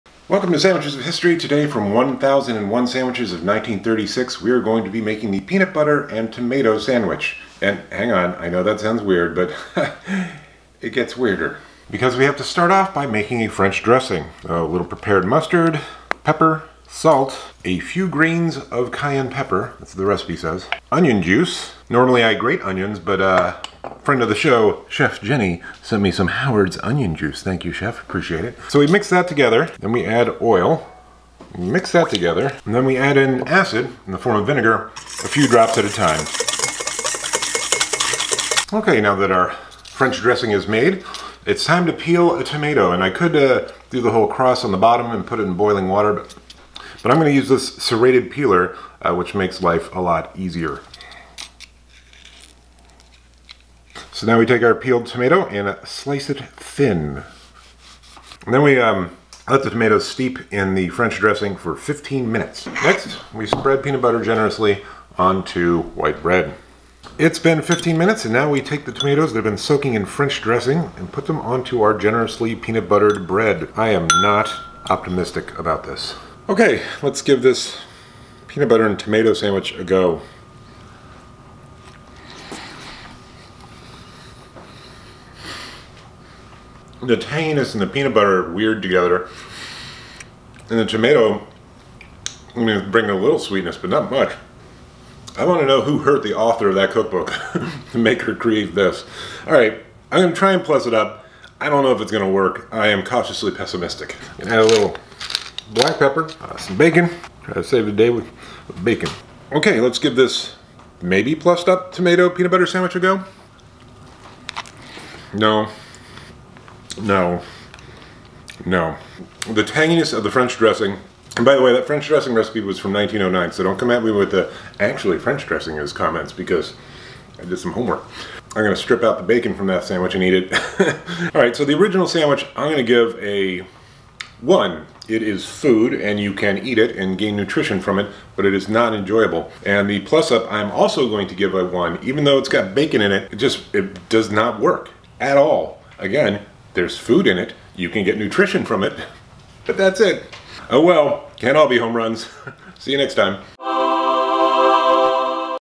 Peanut Butter & Tomato Sandwich (1936) on Sandwiches of History
Ok, I’m not sure who put the author up to this nonsense but the Peanut Butter and Tomato Sandwich from 1936’s “1001 Sandwiches” is absurd on it’s face. Check it out.

Get sneak peeks, exclusive vids, merch, and monthly livestreams, and more!